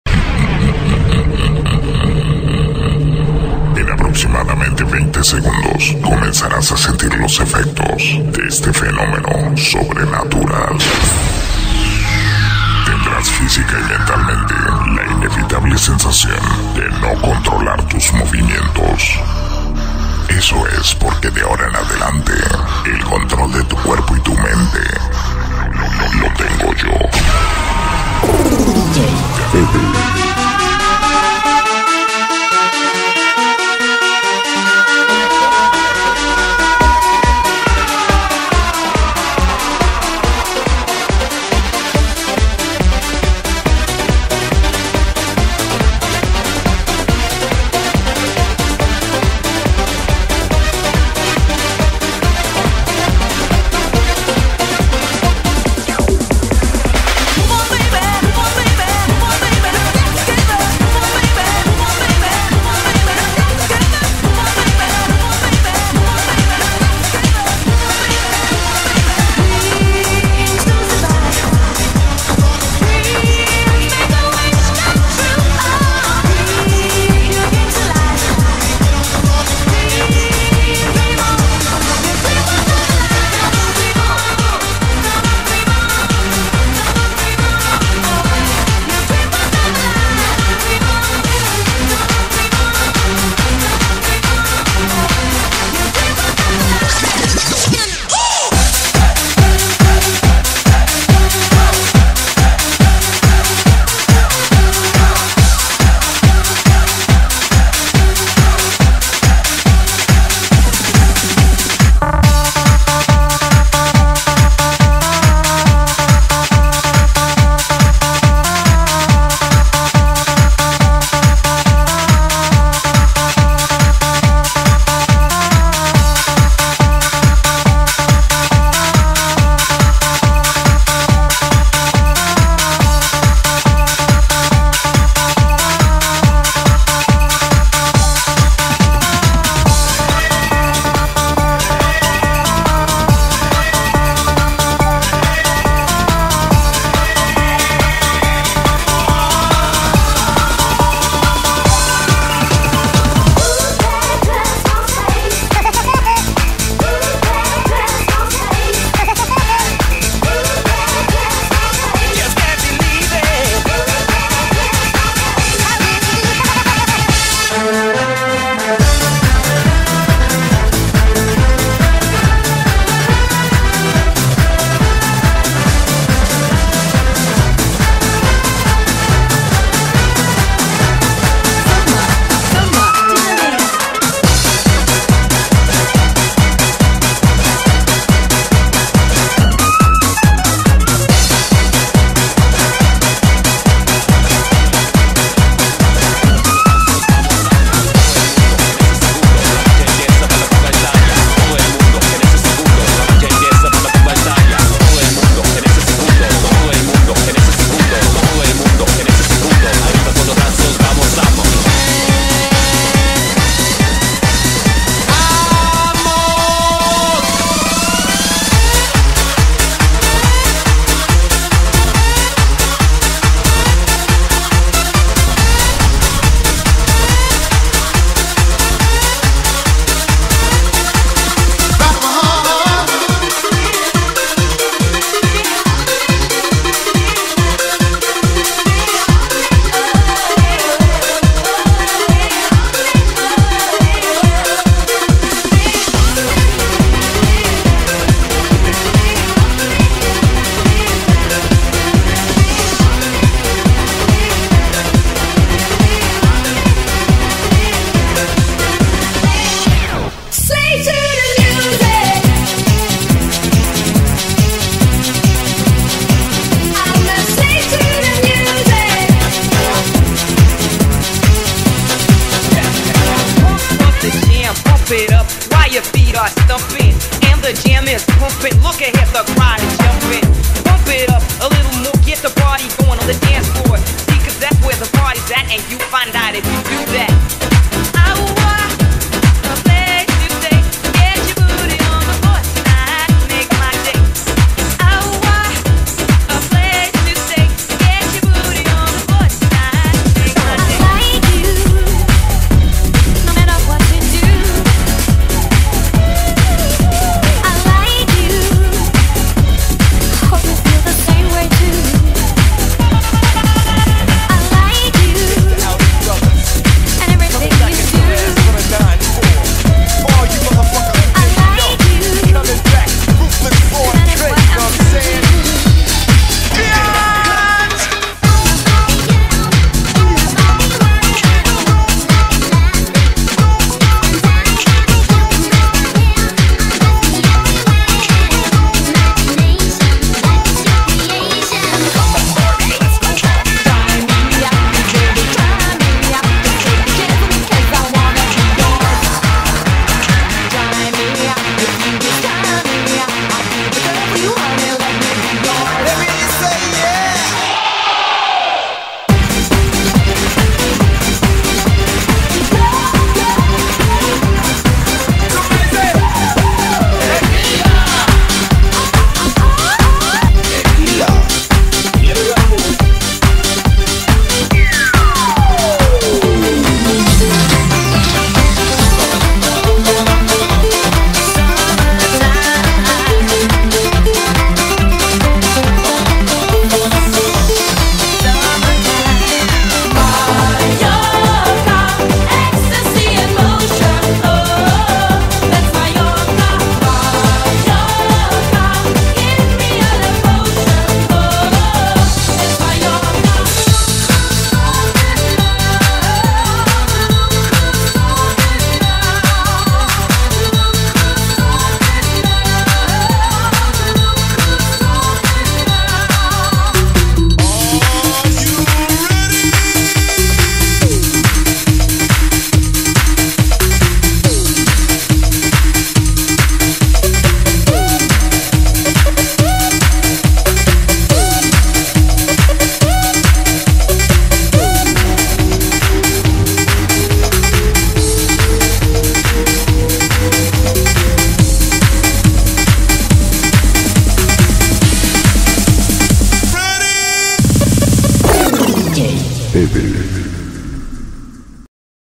MIX TECHNO DANCE 90 2 PP VDJ
Mix Euro Dance de los 90, audio e imagen mezclado por PP VDJ